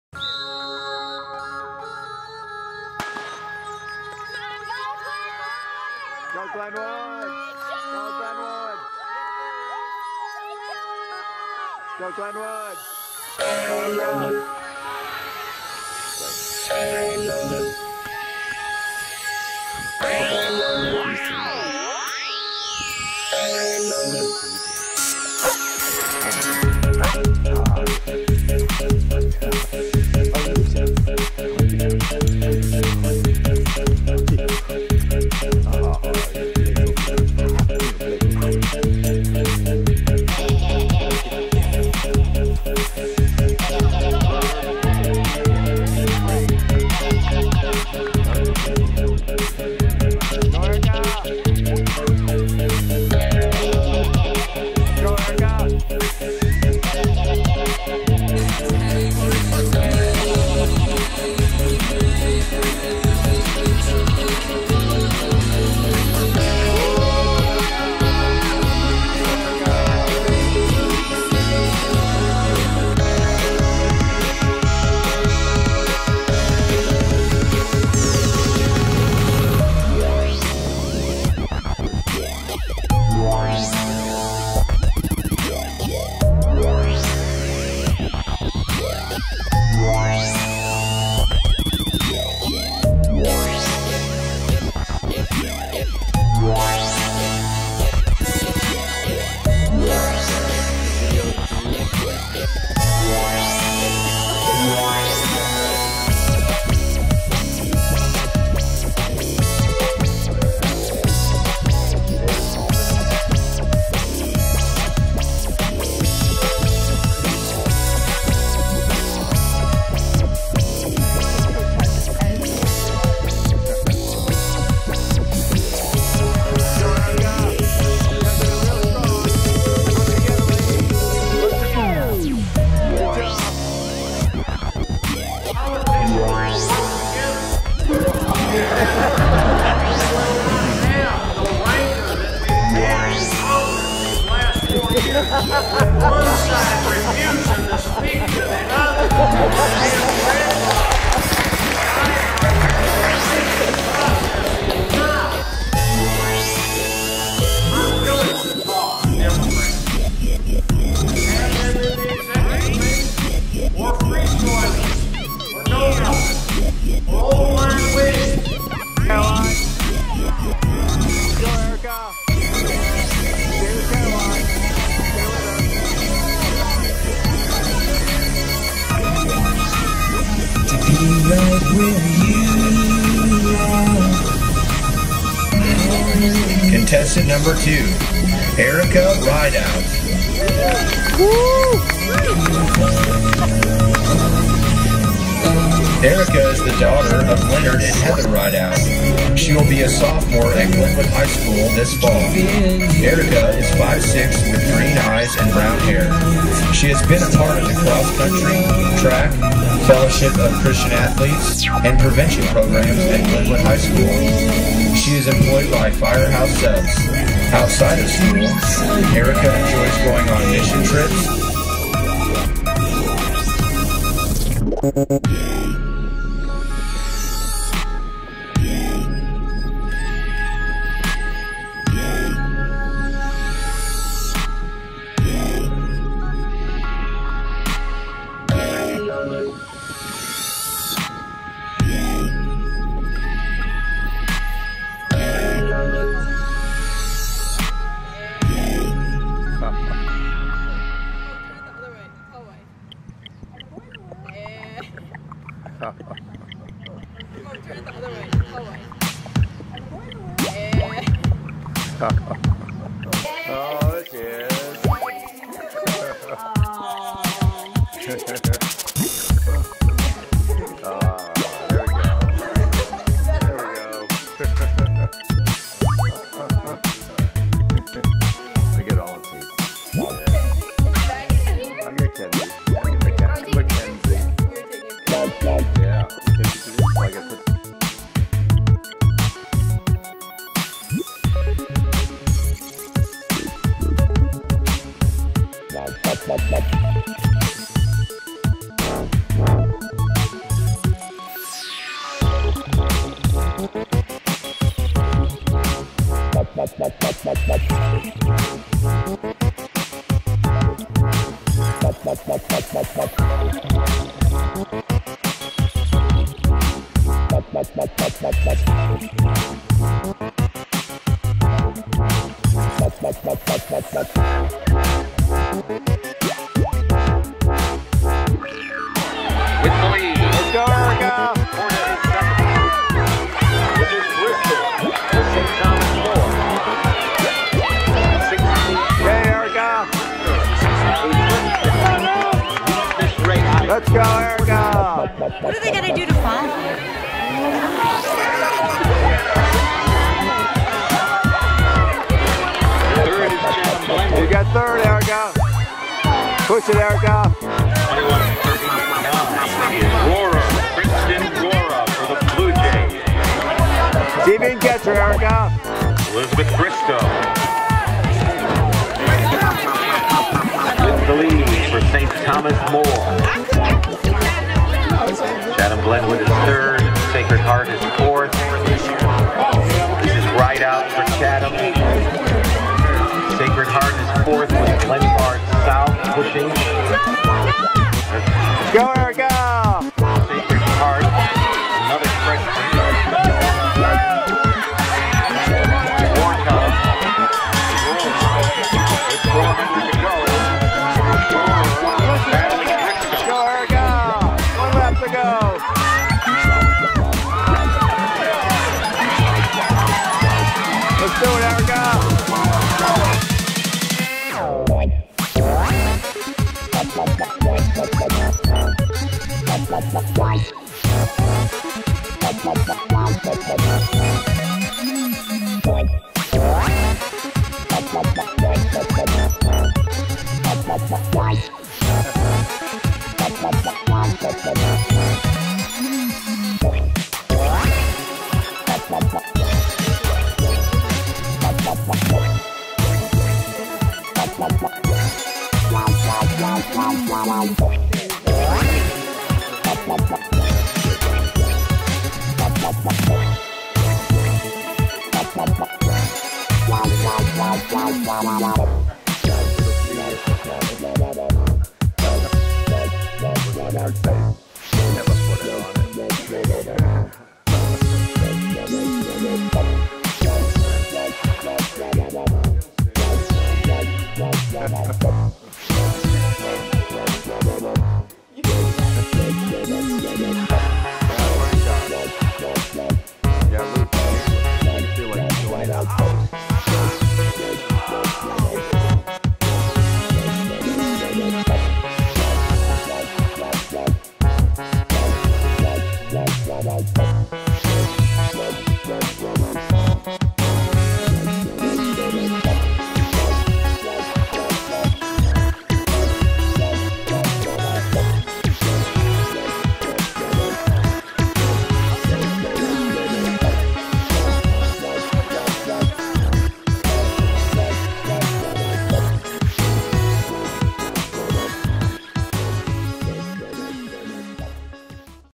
Rideouts 2012
Rideouts in Illinois 2012. Playing around with a few home videos on a winter's day. New original Electronic Dance Music (EDM) composed and recorded by Launchpad Infinity in 2013. Heather, Nick, Erica, Luke, Chloe, Martini, Oliver and Len. Appearances by Bri, Kirsten, Mackenzie, Kerryn, Susan, Lance, the Perko family, George Buss as Abe Lincoln and Superman.